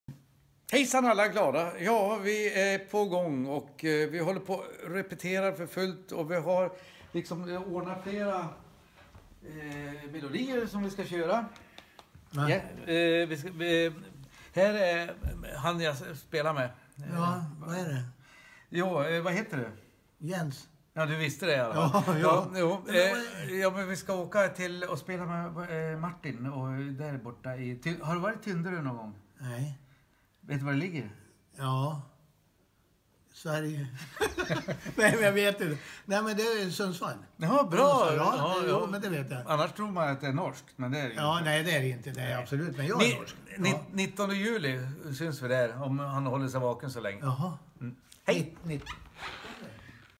Östen och Jens från Östen med resten gästar "Allsång på Tynderö"
Östen Eriksson och Jens Kristensen från Östen med resten gästar "Allsång på Tynderö" den 19 Juli 2017
Kapellmästare Patrik Norman och Allsångsledare Anna och Martin Hanning Häggström.
En Hanning Häggström Produktion